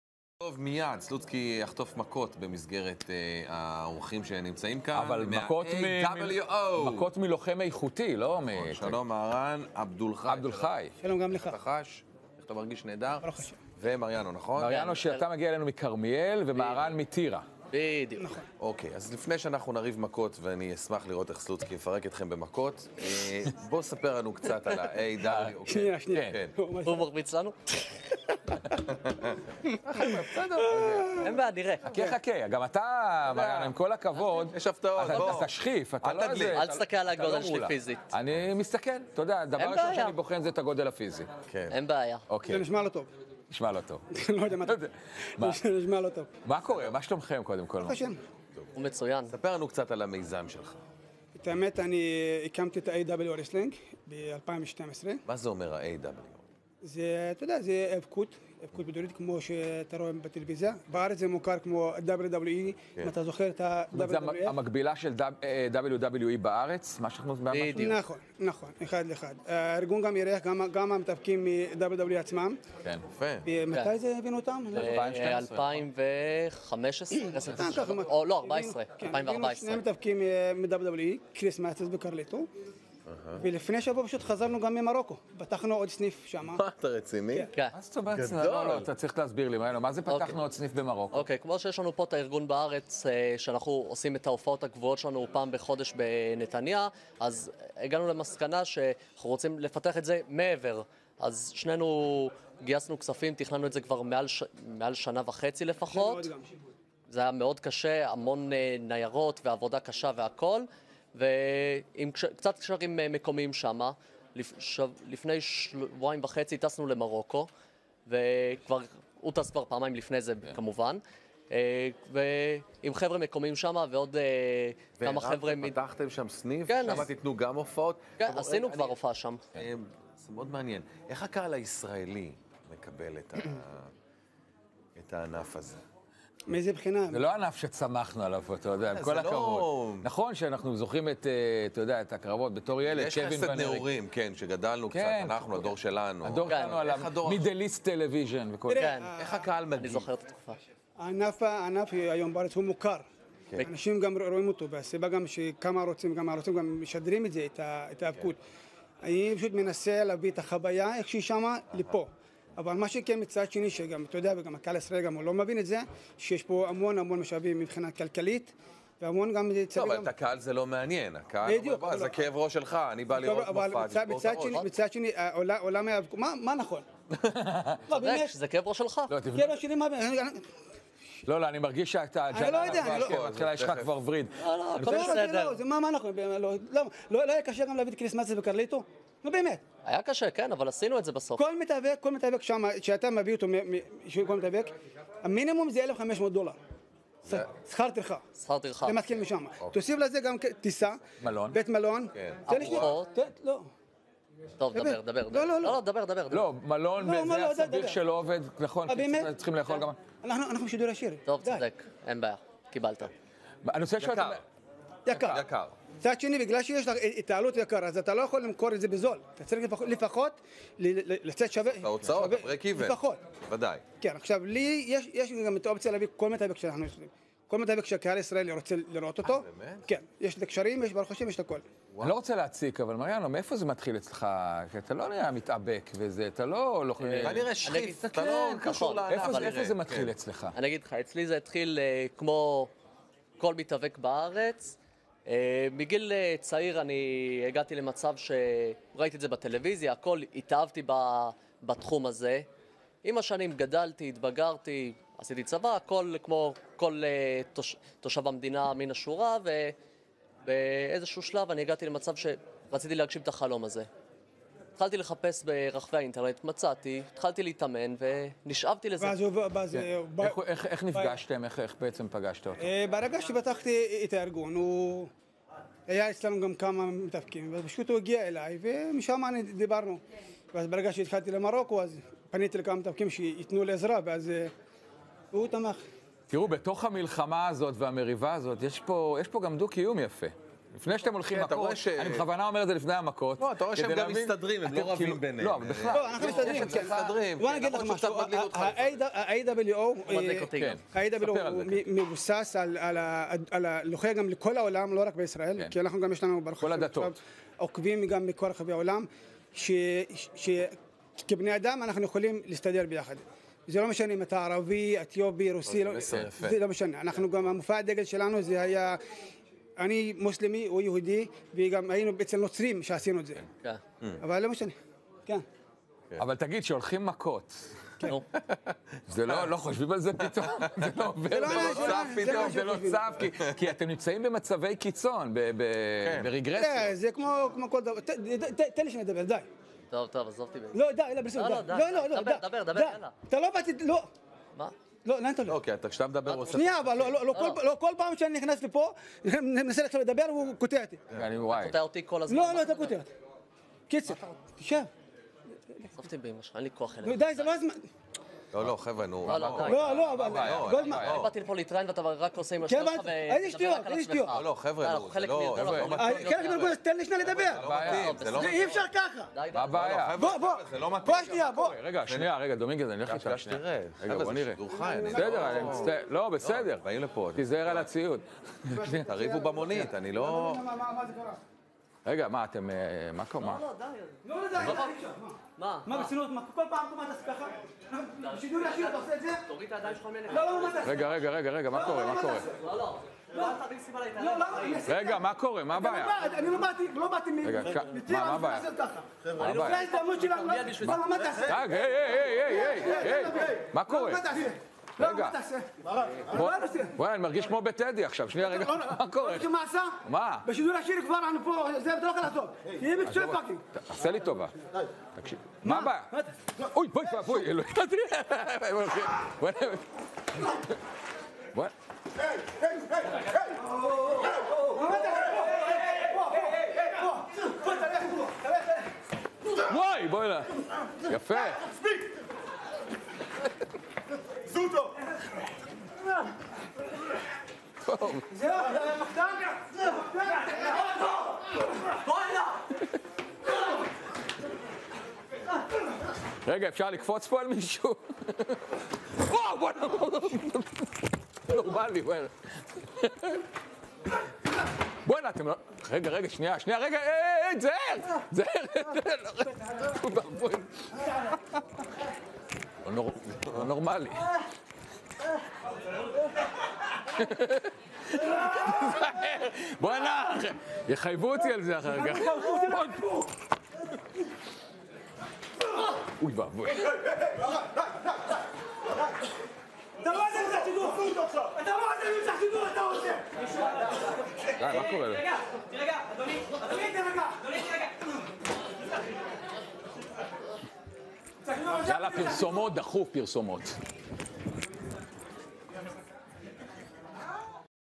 AWO Interview at Sports 5 Live
AWO's the arabian horse and goldman doing an interview for the Sports 5 Channel, and it goes very wrong...